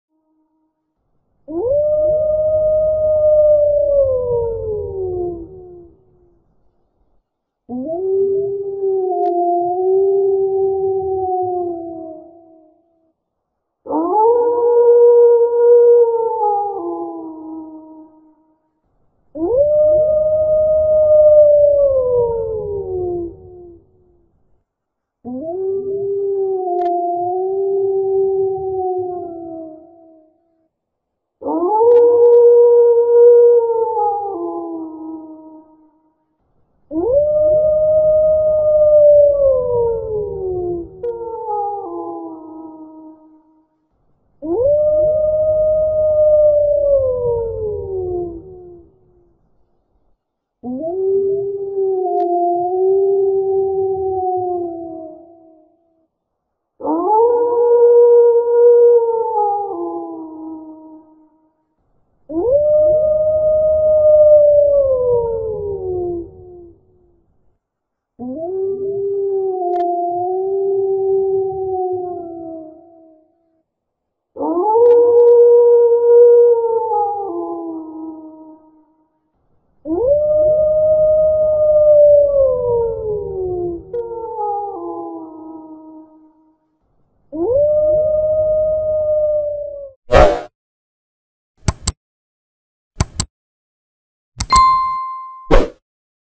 WOLF SOUND - WOLF SOUND EFFECT - NOISE OF WOLF - animal sound
Real WOLF sound effect. Animal sounds recorded with 8D technology. Wear headphones and close your eyes for a better experience.
Animal sound, real animal sound, animal sound for kids, wild animals sound, farm animals sound.

TWIT